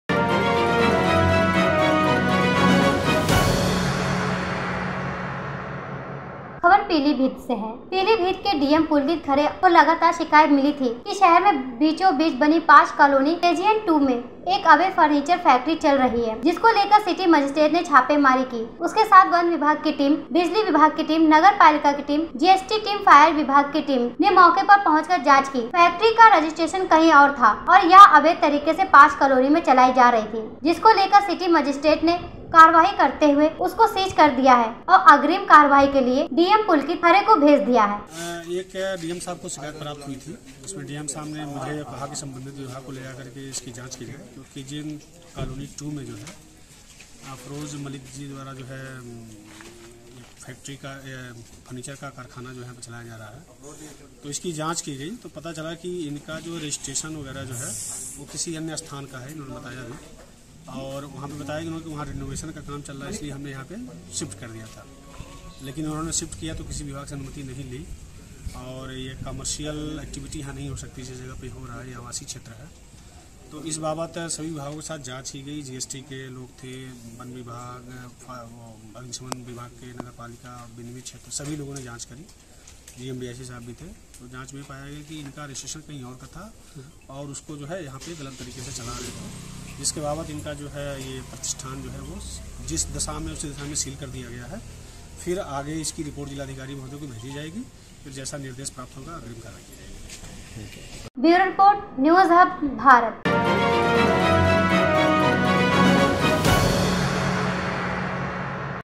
Pilibhit : अवैध फैक्ट्री हुई सीज | NEWS HUB BHARAT
About this video-पीलीभीत के डीएम पुलकित खरे को लगातार शिकायत मिली थी कि शहर के बीचो-बीच बनी पांश कॉलोनी केजीएन टू में एक अवैध फर्नीचर फैक्ट्री चल रही है जिसको लेकर  सिटी मजिस्ट्रेट ने छापेमारी की उनके साथ वन विभाग की टीम बिजली विभाग की टीम नगरपालिका की टीम जीएसटी टीम फायर विभाग की टीम ने मौके पर पहुंचकर जांच की और जांच करवाइए की फैक्ट्री का रजिस्ट्रेशन कहीं और था और यह अवैध तरीके से पाँश कॉलोनी में चलाई जा रही थी जिसको लेकर सिटी मजिस्ट्रेट ने कार्रवाई करते हुए उस को सीज कर दिया है और अग्रिम कार्रवाई के लिए डीएम पुलकित खरे को भेज दिया है |